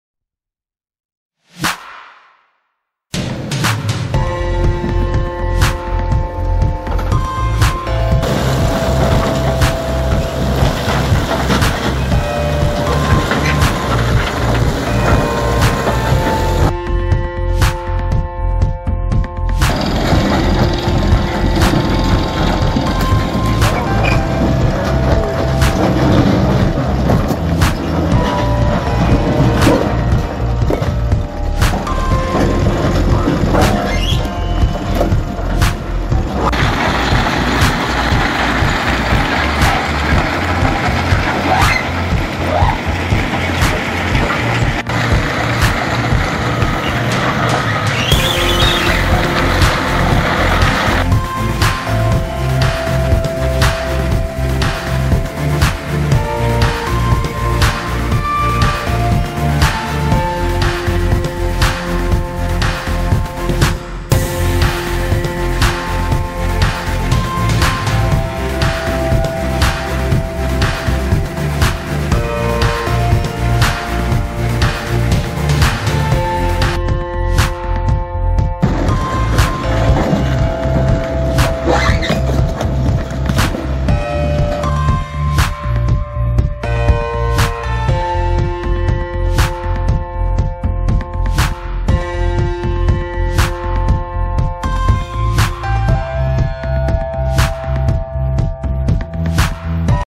MDS M515 and M412 Working together
M412 and M5515 being used by McCourt & Sons in Texas.

Up to 18" Mid Product
Oversize up to 32" (On M515)